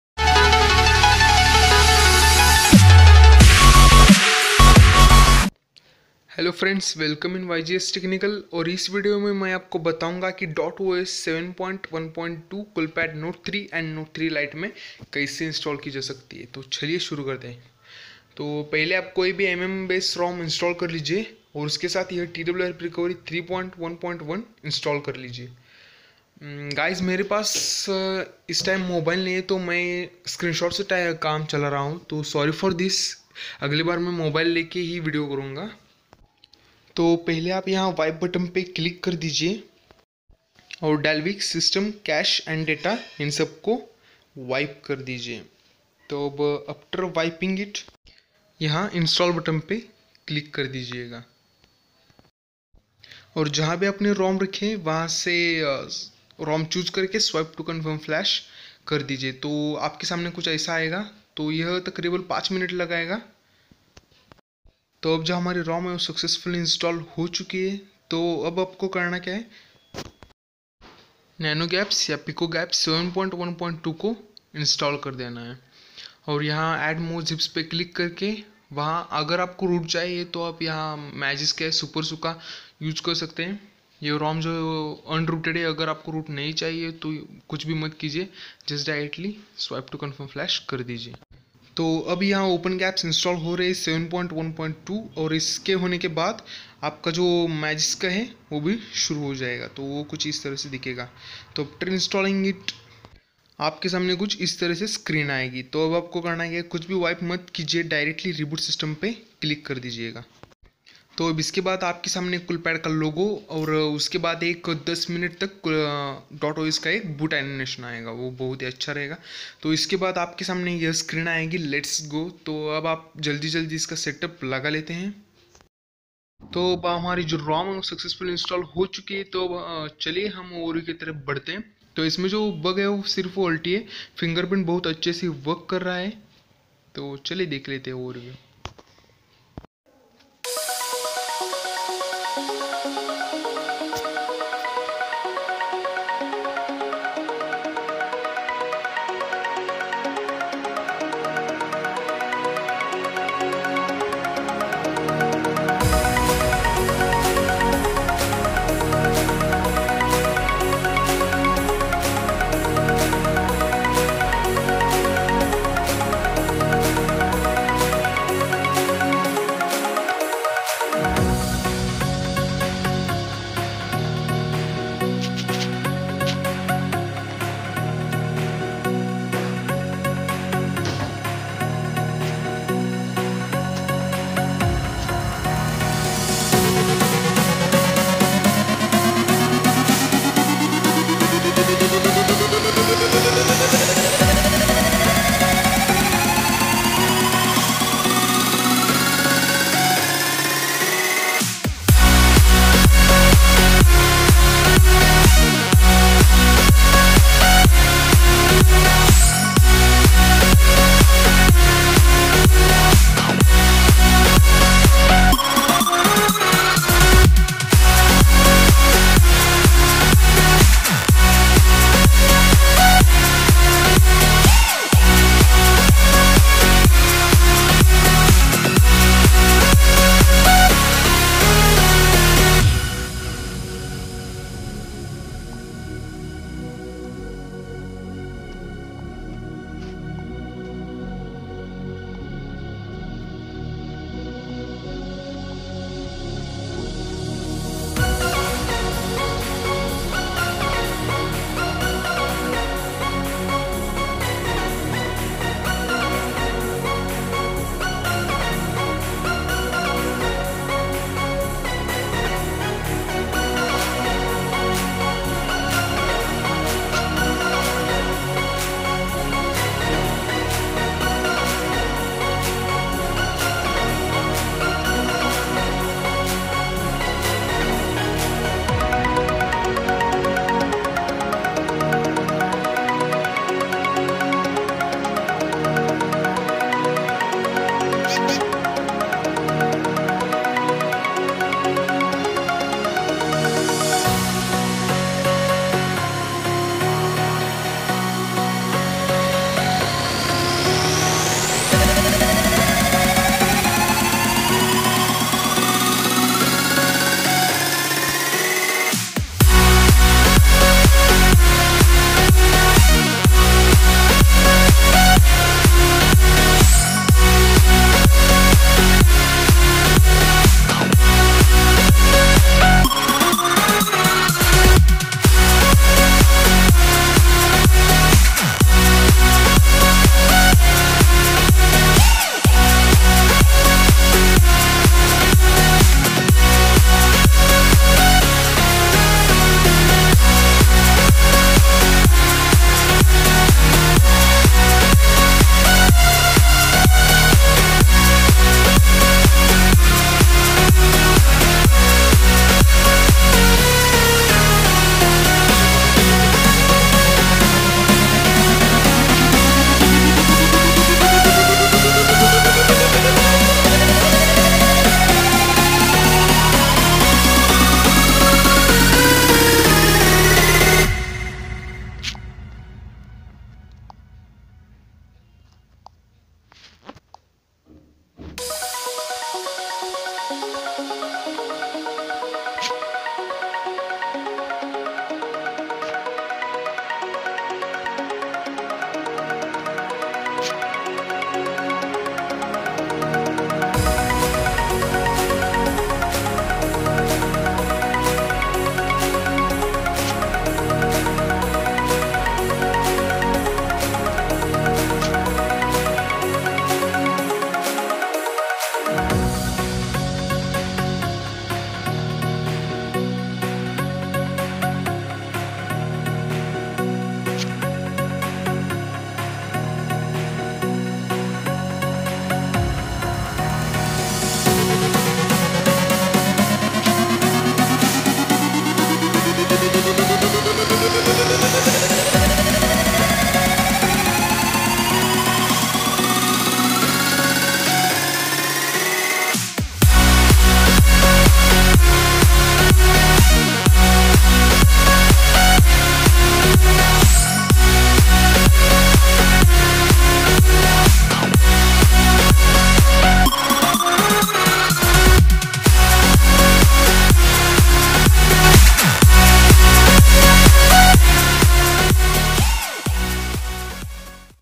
[STABLE] DOT OS for Coolpad Note 3 / Lite
Hello friend I am shubham...

In this video  I am going to show you how to install the dot os on coolpad phones and I also give a overview  About it..

Friends here there is no camera app so install any cam app which u like..

Dot OS v1.1 [UNOFFICIAL]  (Comiled From Source)
So here is guys this rom for you Coolpad note 3 lite users..
This rom is having smooth UI and almost very smooth...

The rom is unrooted😉
If u need root install magisk or SuperSU..

What's  Working:-
WiFi
Bluetooth
Fingerprints
Camera
Ril

What's not working:-
VoLte
Other you tell me...

If you don't get network after flashing the gapps then restart your phone without sim . Now insert your sim card and restart again

Credits:-
MohanCm
Kuber Sharma
Prince chalapathi
Sam
Sougata Talukdar

New Dot OS ( Flash This)
Link for CN3L:-

YGS Technical official WhatsApp group:-